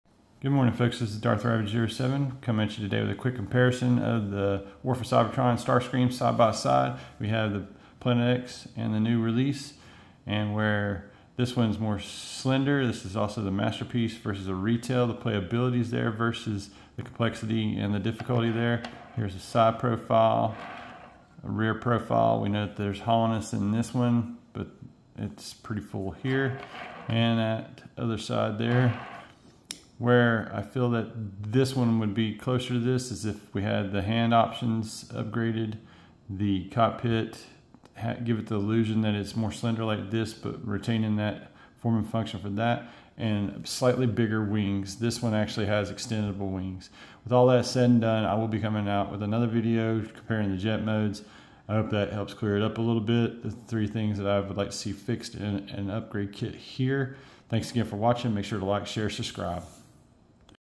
Comparison: SS GE (06) vs Planet X WFC Starscream
Quick Robot Mode Comparison GE (06) vs Planet X War for Cybertron Starscream!